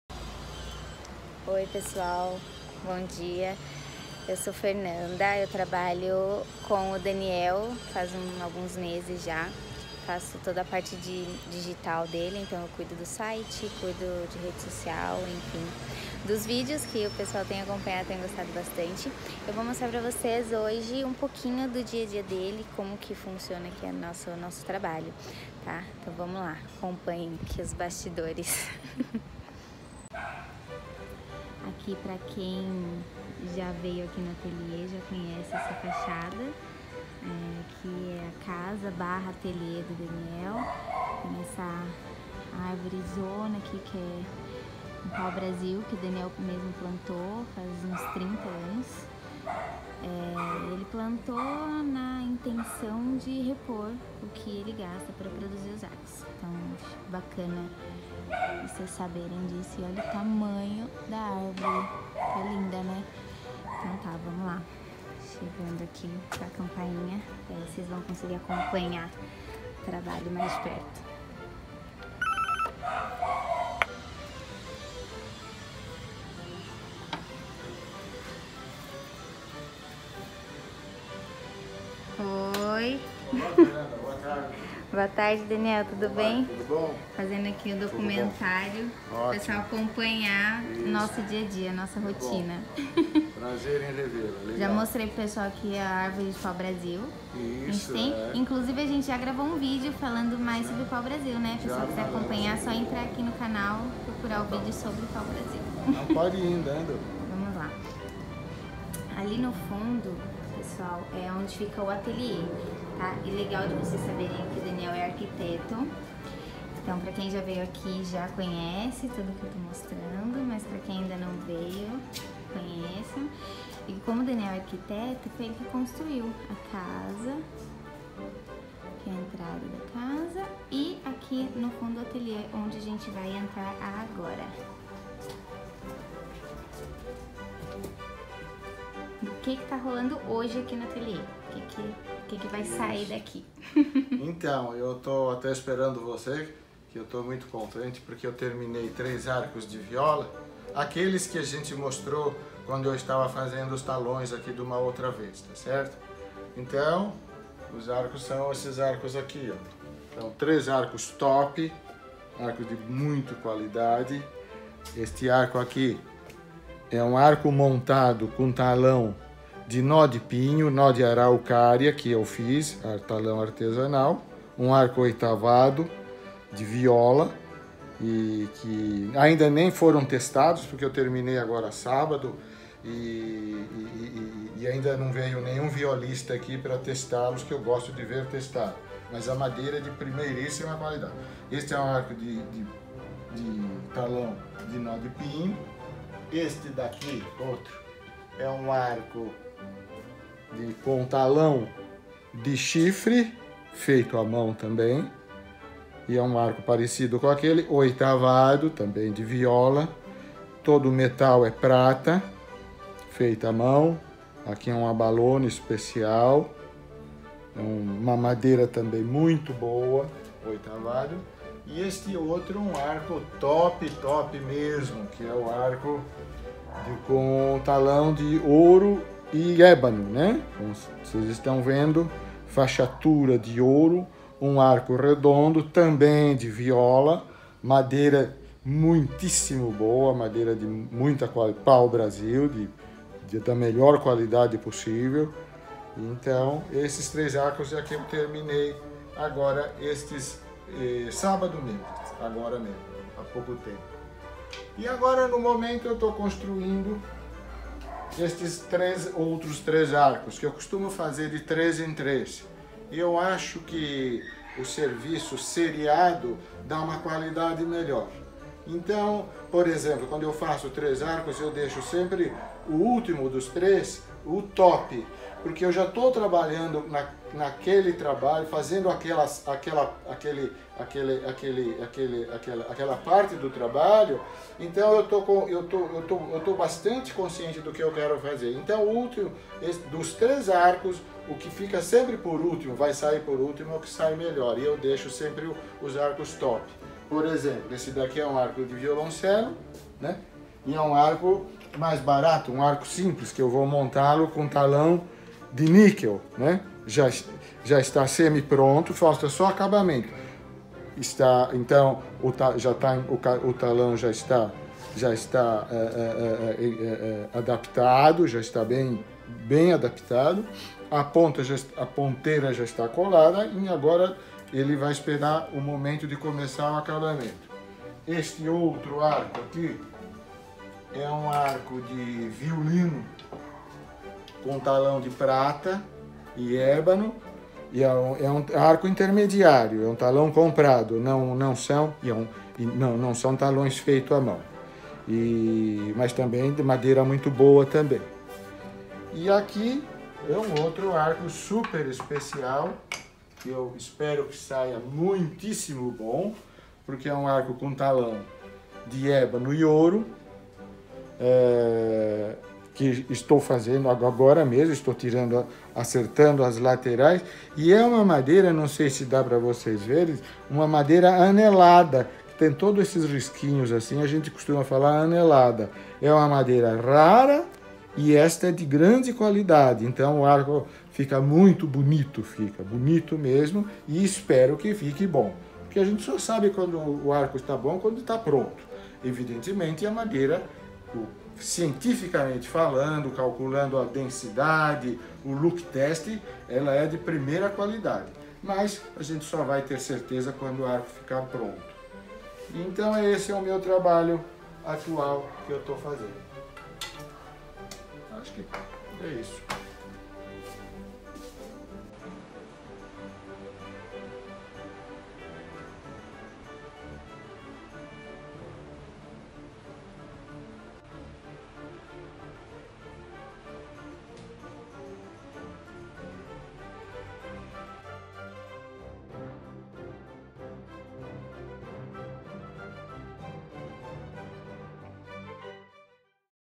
Bastidores do Atelier Lombardi Arcos - Arcos para Instrumentos de Corda
Arcos para todos os 4 instrumentos de corda: violino, viola, violoncelo e contra-baixo. Os arcos são fabricados artesanalmente com especial atenção ao chamado "ponto de equilíbrio". Os arcos fabricados por Daniel Lombardi seguem criterioso método desde a escolha da madeira.
Os arcos possuem 3 qualidades: níquel, prata e ouro.